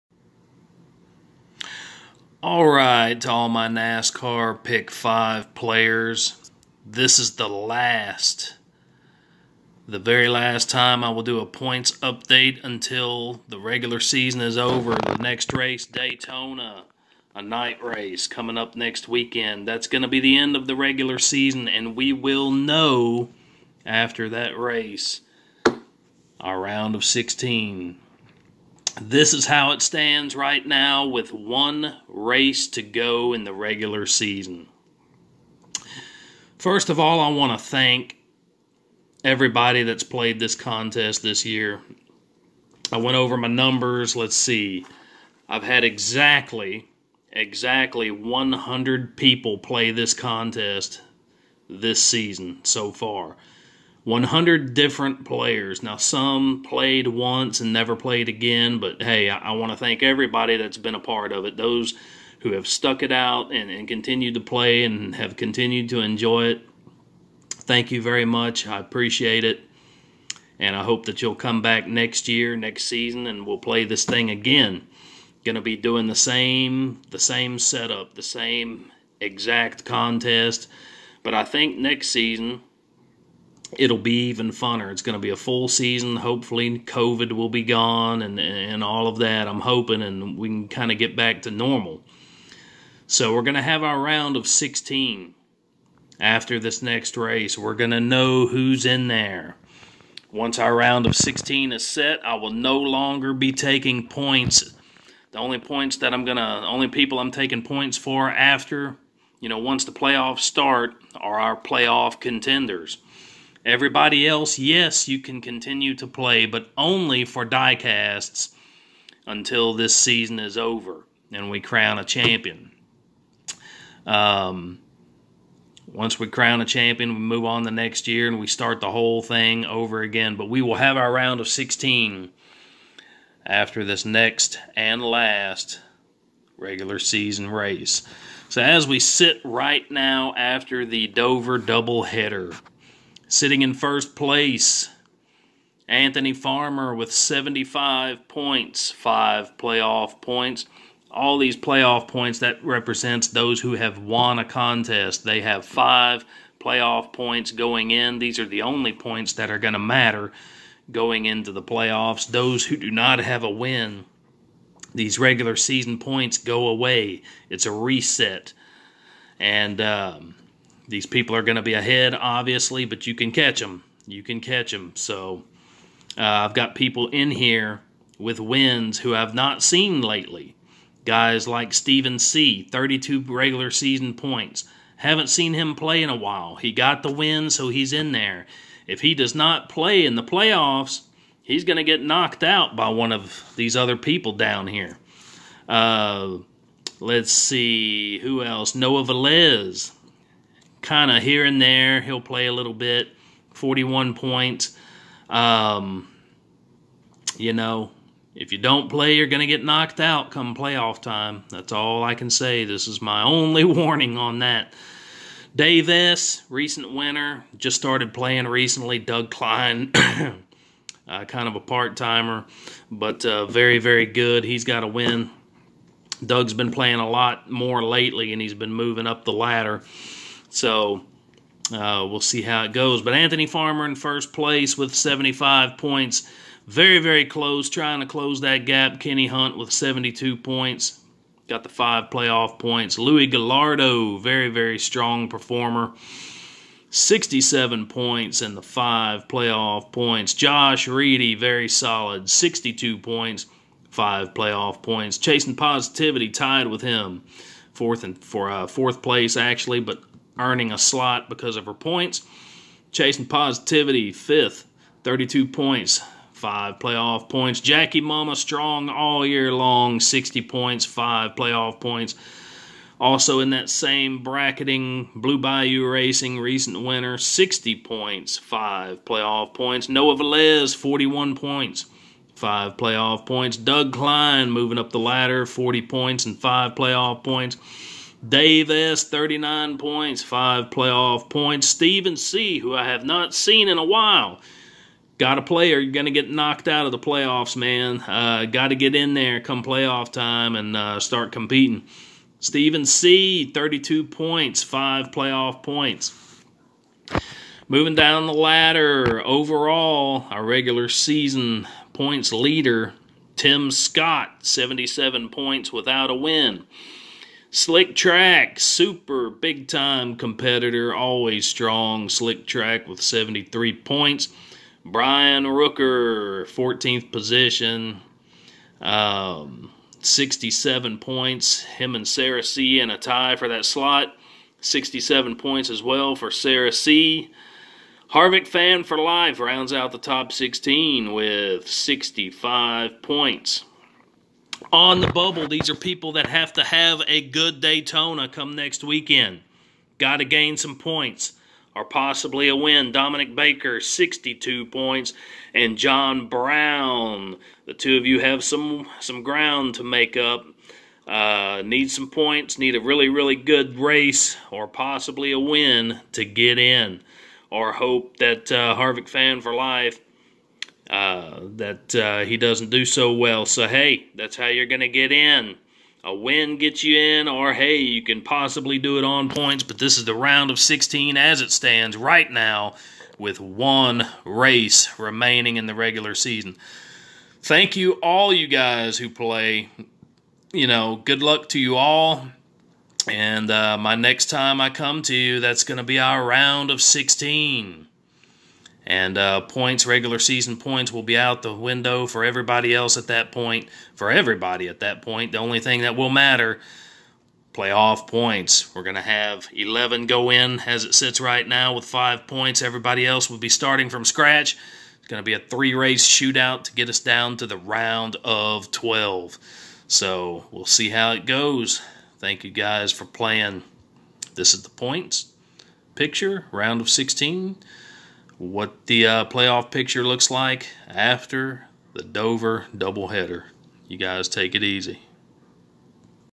To ALL *pick 5* Players. Points standings after Dover (#2)
All of my NASCAR Pick 5 players... This is the point standings as we sit right now with One race remaining in the regular season. My next points update will show who has made it to the playoffs officially and who is out. If you do not make the playoffs you can continue to come back every week and play for Diecast, but points will only be counted for those in the playoffs at that point. Next season I will be back and we will start this thing all over again. Thank you all for playing and as always good luck to you all!